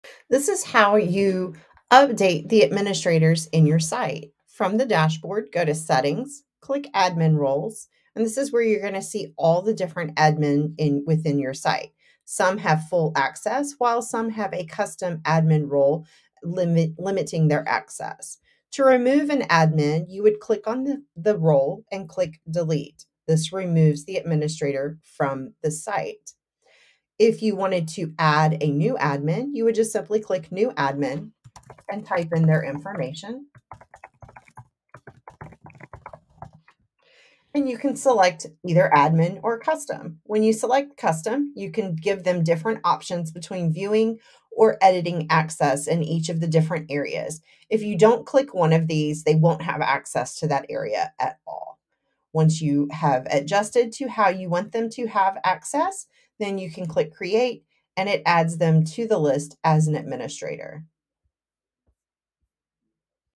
Adding and Removing Administrators
This is how to add/remove administrators within your Givebacks site.